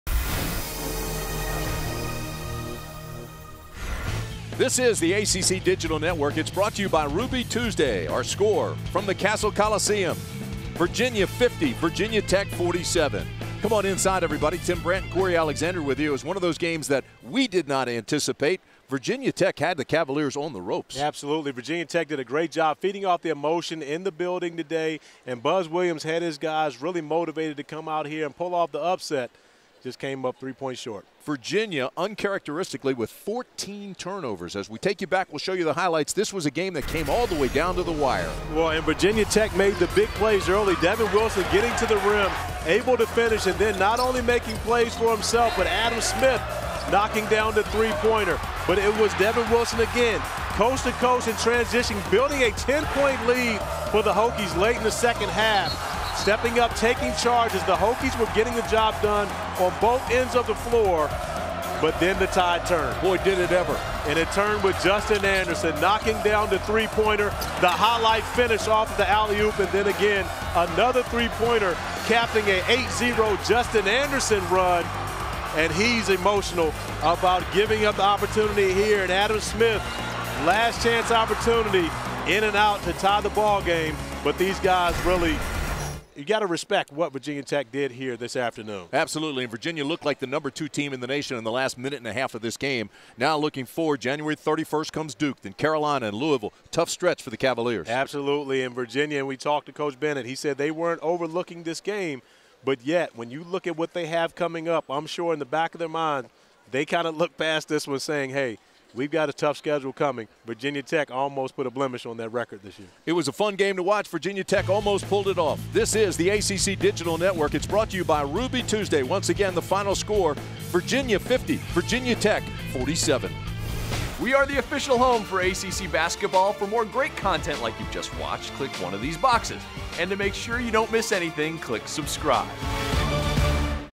Virginia vs Virginia Tech | 2014-15 ACC Men's Basketball Highlights
Justin Anderson scored 12 points and hit two second-half 3-pointers to lead #2 Virginia to a comeback win against Virginia Tech 50-47 in Blacksburg.  Adam Smith scored 15 points for the Hokies who led by as many as 10 points after halftime.  The Cavaliers outscored the Hokies 10-2 in the final four minutes of the game to survive the upset bid.